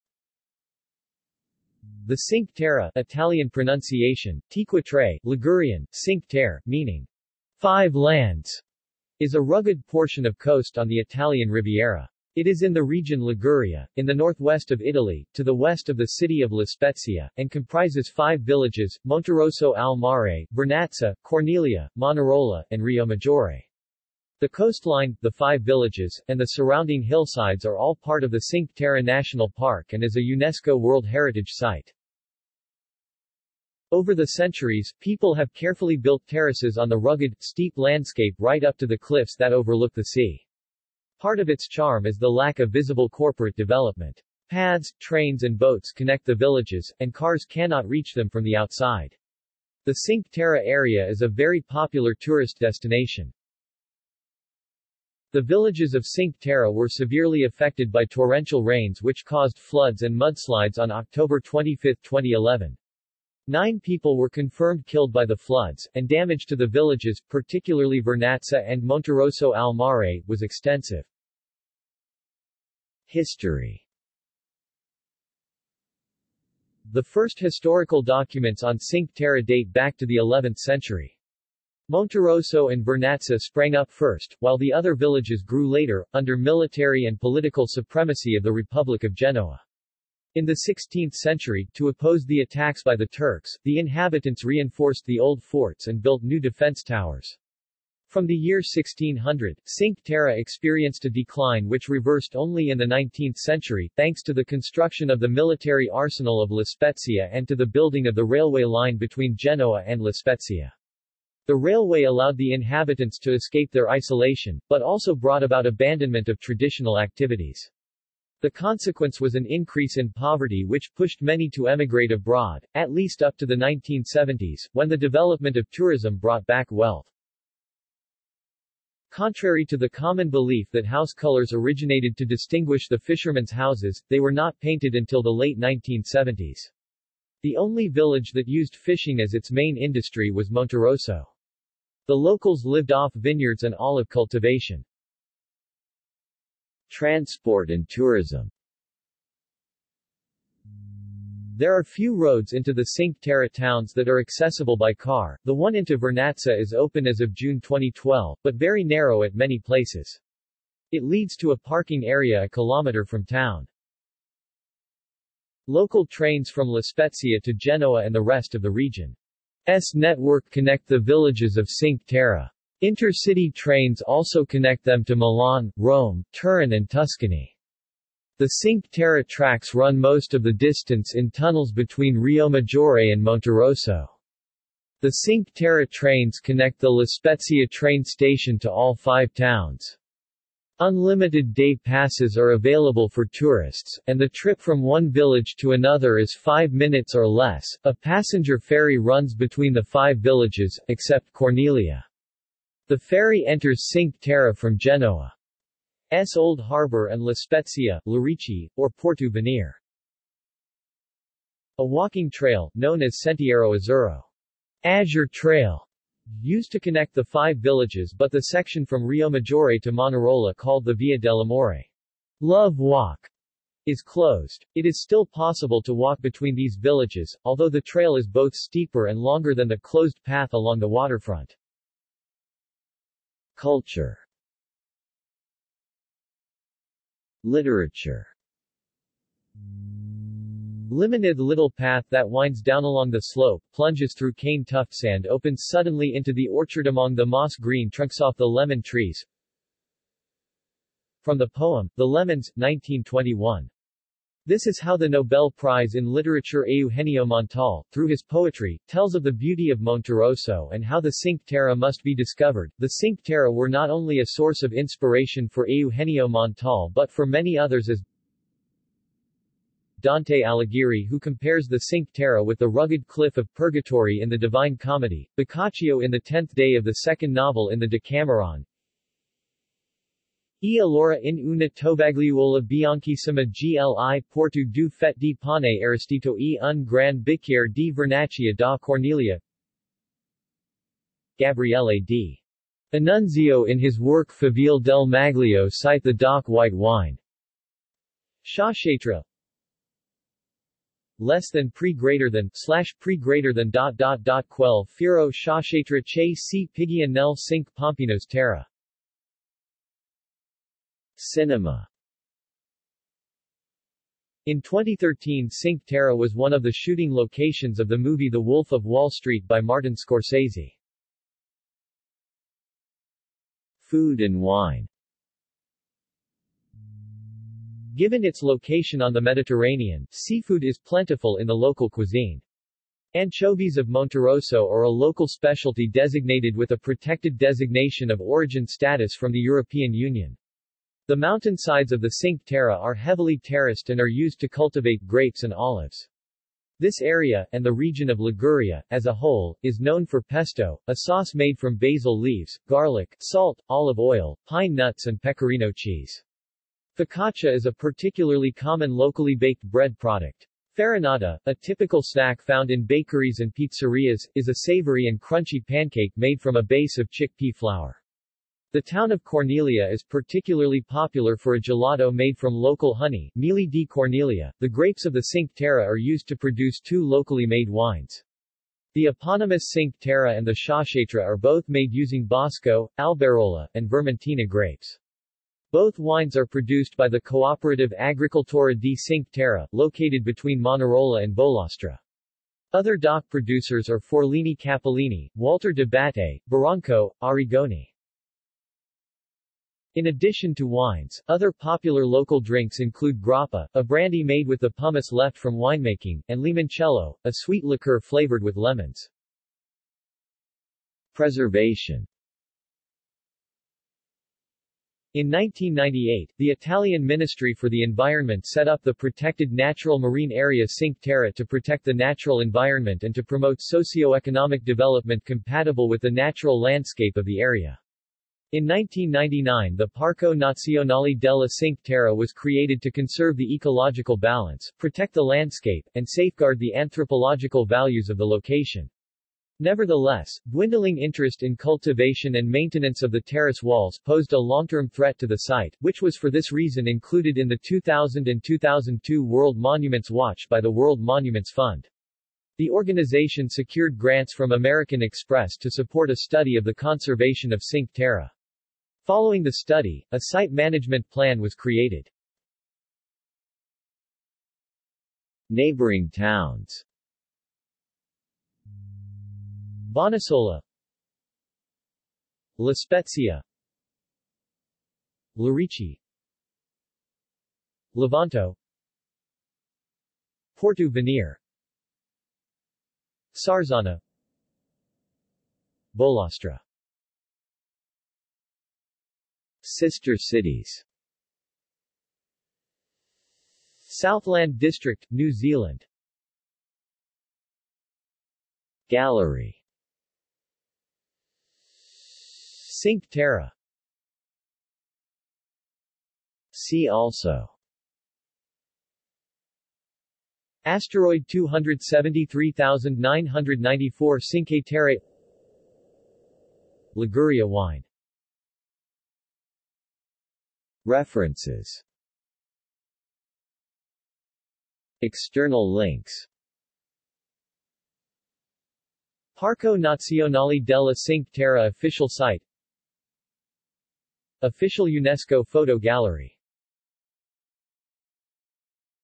Cinque Terre | Wikipedia audio article
This is an audio version of the Wikipedia Article:
Cinque Terre

SUMMARY
The Cinque Terre (Italian pronunciation: [ˈtʃiŋkwe ˈtɛrre]; Ligurian: Çinque Tære, meaning "Five Lands") is a rugged portion of coast on the Italian Riviera. It is in the region Liguria, in the northwest of Italy, to the west of the city of La Spezia, and comprises five villages: Monterosso al Mare, Vernazza, Corniglia, Manarola, and Riomaggiore. The coastline, the five villages, and the surrounding hillsides are all part of the Cinque Terre National Park and is a UNESCO World Heritage Site.
Over the centuries, people have carefully built terraces on the rugged, steep landscape right up to the cliffs that overlook the sea.  Part of its charm is the lack of visible corporate development. Paths, trains and boats connect the villages, and cars cannot reach them from the outside. The Cinque Terre area is a very popular tourist destination.
The villages of Cinque Terre were severely affected by torrential rains which caused floods and mudslides on October 25, 2011. Nine people were confirmed killed by the floods, and damage to the villages, particularly Vernazza and Monterosso al Mare, was extensive.